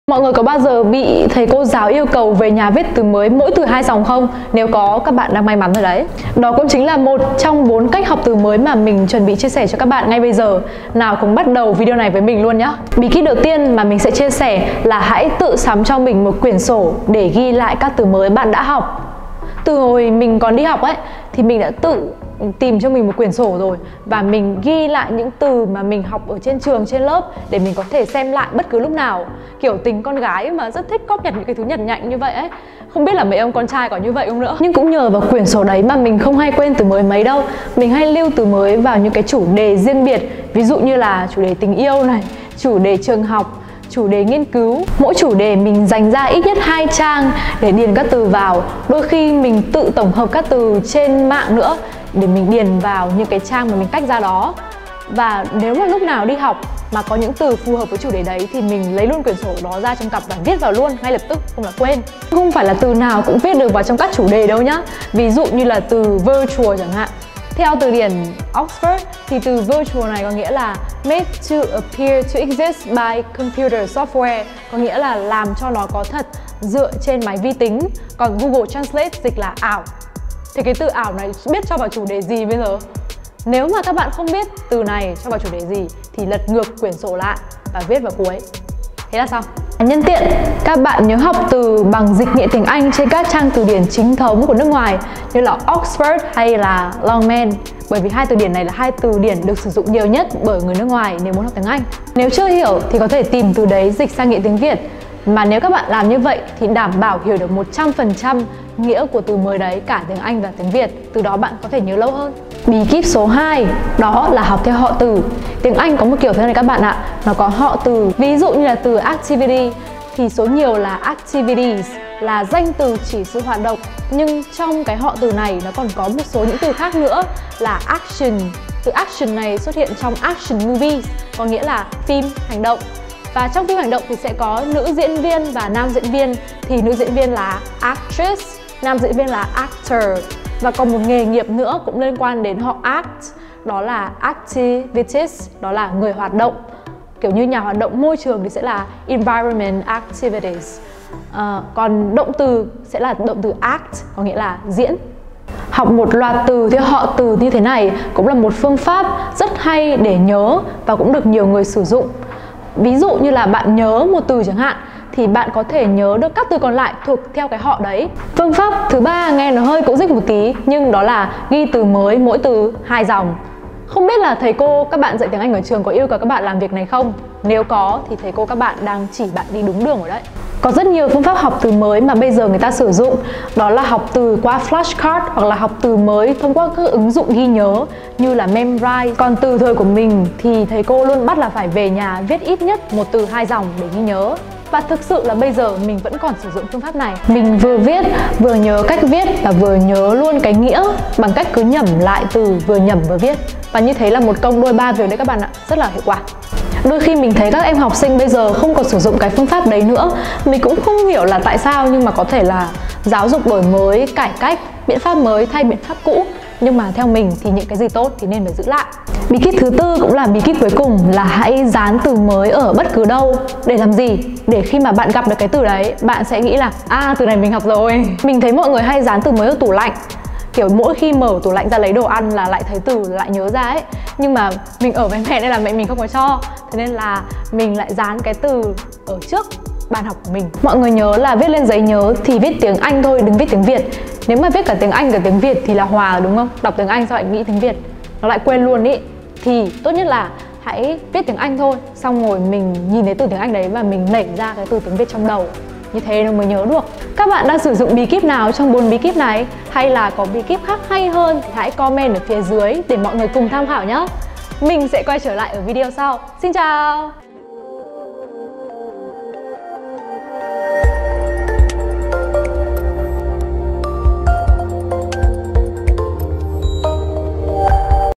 4 mẹo học từ vựng độc nhất vô nhị | IELTS ALES
4 mẹo học từ vựng độc nhất vô nhị | IELTS ALES

Nếu chưa tìm ra phương pháp học cho bản thân thì bạn thử tham khảo 4 cách học từ vựng tiếng Anh độc nhất vô nhị này. Chắc chắn bạn sẽ nhận thấy trình độ tiếng Anh của bản thân thay đổi rõ rệt sau khi biết được các cách học từ vựng tiếng Anh hiệu quả dưới đây.

Thứ nhất: Có một quyển sổ tổng hợp từ mới

Nếu bạn thấy phấn khởi với ý tưởng ghi chép lại bằng bút giấy, hãy sử dụng một cuốn sổ tay để lưu trữ tất cả những thông tin cần thiết. Bạn có thể sử dụng những cuốn sổ tay từ vựng tiếng Anh sắp xếp theo thứ tự bảng chữ cái hoặc mã màu riêng để có thể dễ dàng quản lý hơn, nhưng bạn nên nhớ một điều rằng yếu tố quan trọng là phải dễ tìm kiếm.
Thứ hai: Học theo họ từ
Đừng bỏ qua những “người họ hàng” của các từ mà bạn định học nhé. Các bạn sẽ có thể mở rộng vốn từ của mình đáng kể nếu ngoài việc học từ, các bạn học thêm cả các loại từ khác nhau của chúng nữa. Một trong những cách giúp các bạn học nhanh những loại từ khác nhau của từ đó chính là nhớ được các hậu tố của từ. Phần lớn các hậu tố khi được thêm vào từ gốc sẽ thay đổi loại từ của từ gốc đó. Vậy chỉ cần nhớ được từ gốc và các hậu tố, các bạn đã có thể mở rộng vốn từ của mình gấp 3, 4 lần mà không cần phải tốn quá nhiều công sức rồi.
Thứ ba: Ghi từ mới mỗi từ hai dòng
Mỗi từ  2 - 4 dòng, mỗi dòng 5 chữ – đó là cách học từ kinh điển của rất nhiều học sinh sinh viên Việt Nam. Việc lặp lại sẽ giúp bạn ghi nhớ từ vựng nhanh và giúp bạn nhớ lâu hơn từ vựng đó. Với 1 từ, bạn gặp từ đó khoảng 10-20 lần là nhớ từ vĩnh viễn. Việc bạn không nhớ từ là do bạn chưa chịu ôn tập đủ và gặp từ đủ số lần mà thôi.

XEM THÊM VIDEO BỔ ÍCH KHÁC ĐƯỢC CHIA SẺ HỌC IELTS CÙNG ALES